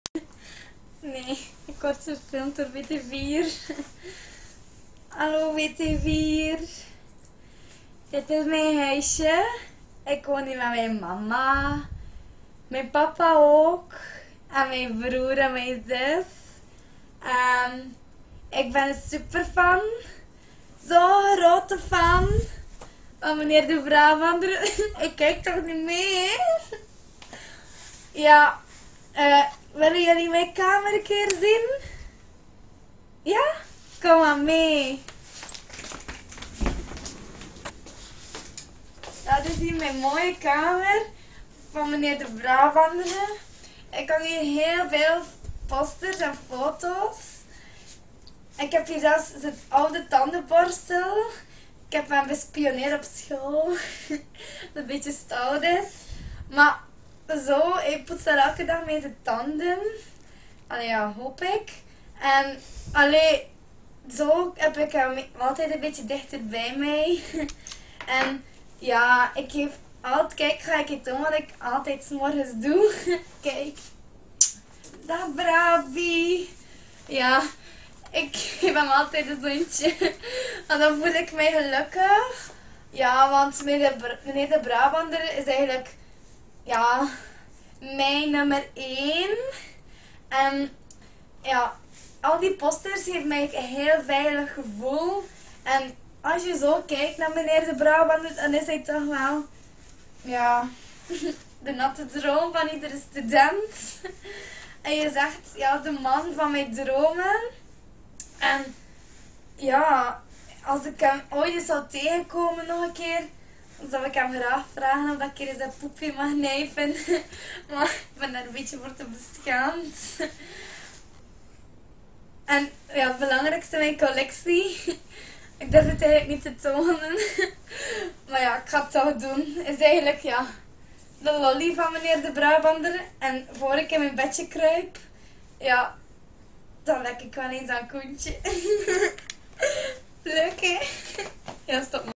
Superfans parodie - Brabie
Een parodie van superfans.
Deze superfan is dus fan van Mr. Brabie de beste leerkracht op haar school.